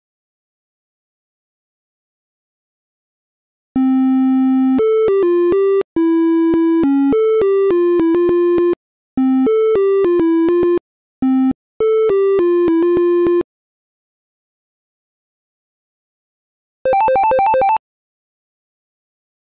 Steven Universe: Garnet Gem Fusion in MakeCode @adafruit @johnedgarpark @cartoonnetwork
Garnet's Gem Fusion on a Circuit Playground Express in MakeCode's simulator! Learn Guide coming soon.